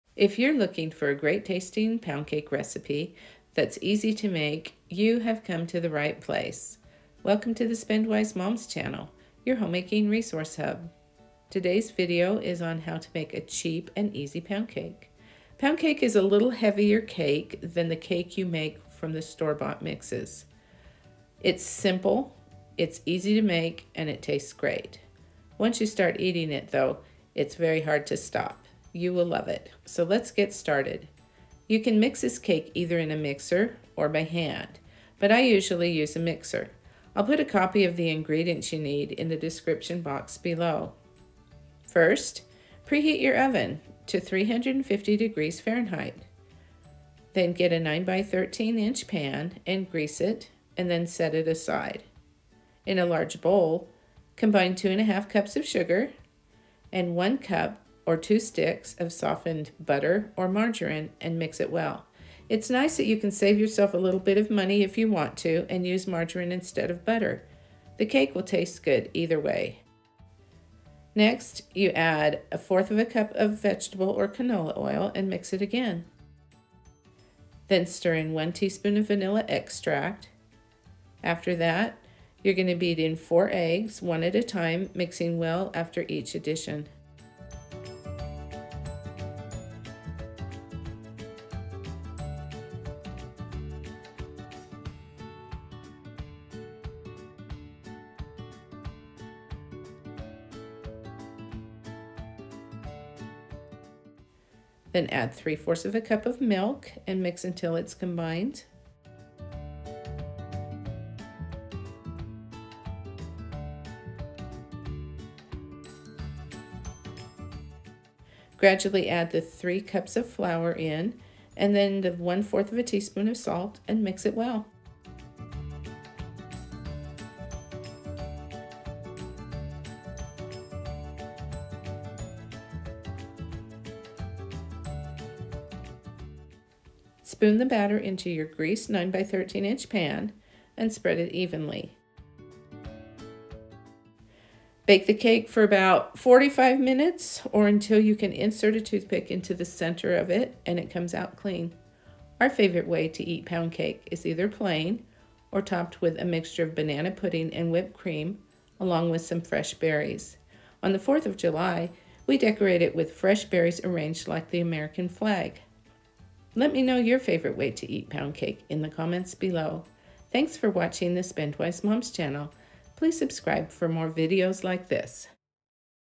Cheap & Easy Pound Cake: Tastes Great!
In this video, we share a great-tasting pound cake recipe that is cheap and easy to make.

Ingredients:

2 1/2 cups sugar
1 cup (2 sticks) softened butter or margarine
1/4 cup vegetable or canola oil
1 teaspoon vanilla extract
4 eggs
3/4 cup milk
3 cups all-purpose flour
1/4 teaspoon salt